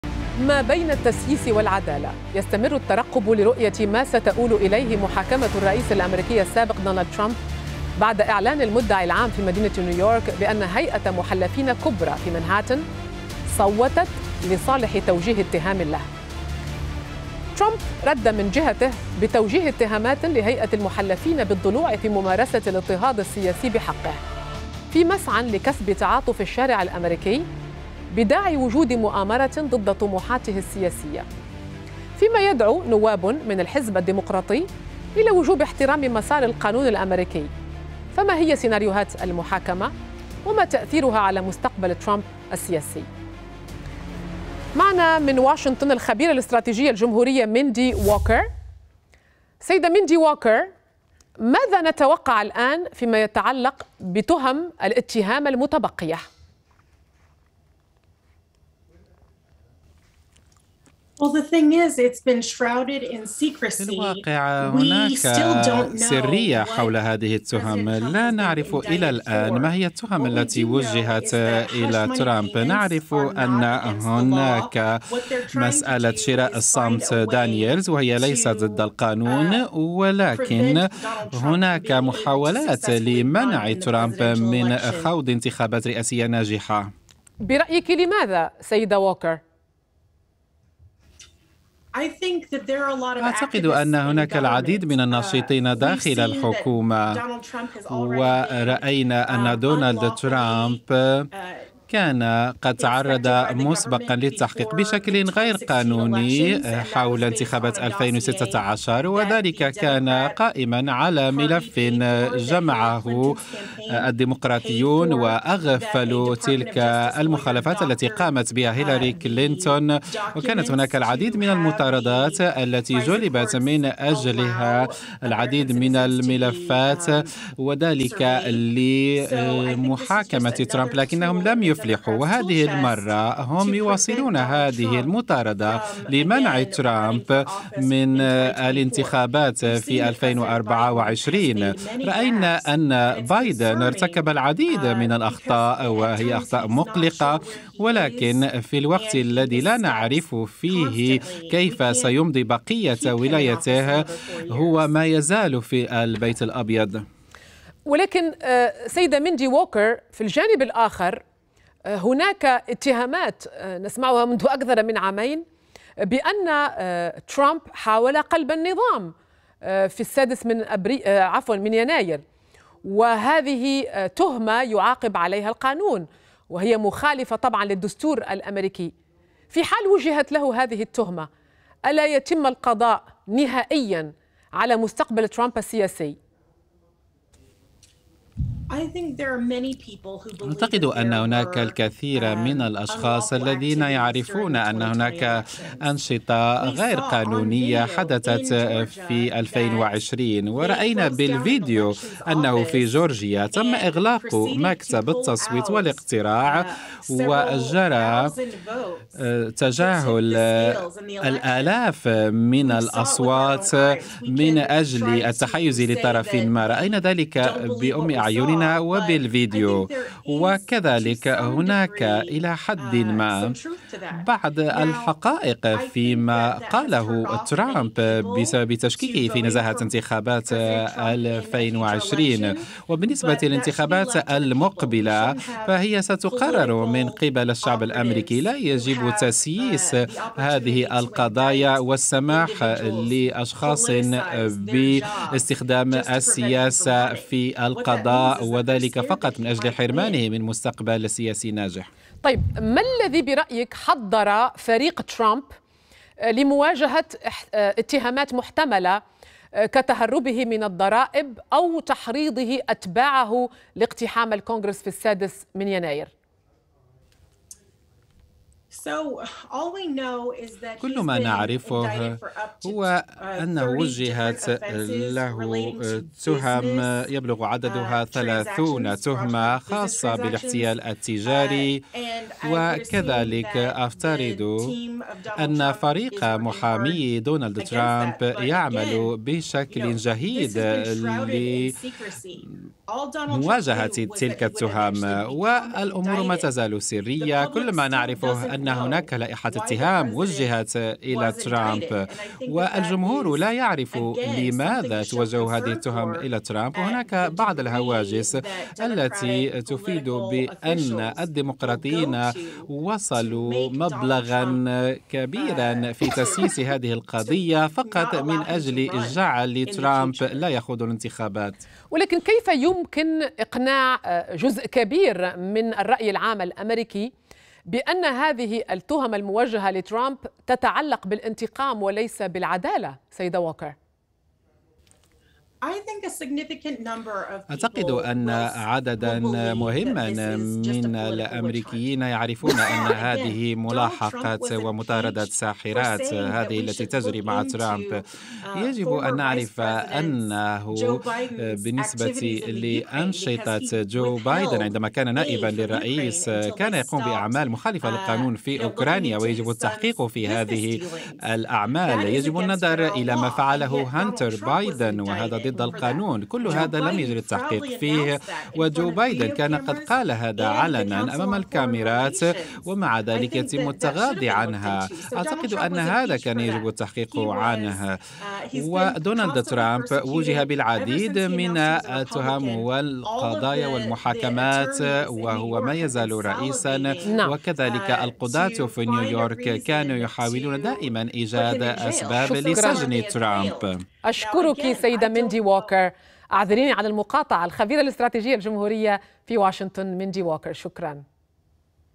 ترقب لسيناريوهات محاكمة الرئيس الأميركي السابق دونالد ترامب
ما بين التسييس والعدالة ... يستمر الترقب لرؤية ما ستؤول إليه محاكمة الرئيس الأميركي السابق دونالد ترامب بعد إعلان المدعي العام في مدينة نيويورك أن هيئة محلفين كبرى في مانهاتن صوتت لصالح توجيه اتهام له.
ترامب رد من جهته بتوجيه اتهامات لهيئة المحلفين بالضلوع في ممارسة الاضطهاد السياسي بحقه، في مسعى لكسب تعاطف الشارع الأميركي بداعي وجود مؤامرة ضد طموحاته السياسية، فيما يدعو نواب من الحزب الديمقراطي إلى وجوب احترام مسار القانون الأميركي... فما هي سيناريوهات المحاكمة؟ وما تأثيرها على مستقبل ترامب السياسي؟